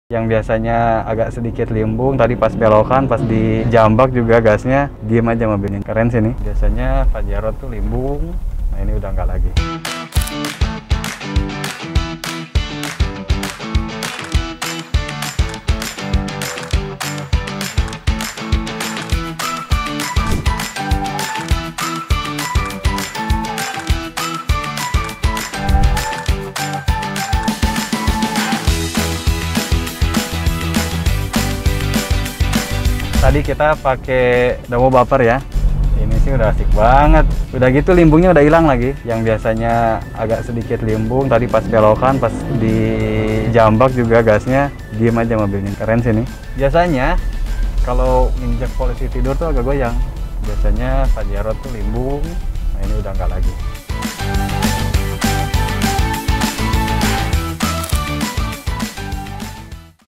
Mantan Pemain Timnas Indonesia Membuktikan Bahwa Domo Buffer Yang Terbaik
Halo Domolovers, kali ini giliran Mang Atep , mantan pemain Timnas Indonesia dan Persib Bandung yang mencoba produk Domo Buffer dari Dokter Mobil.

Domo Buffer memberikan garansi seumur hidup.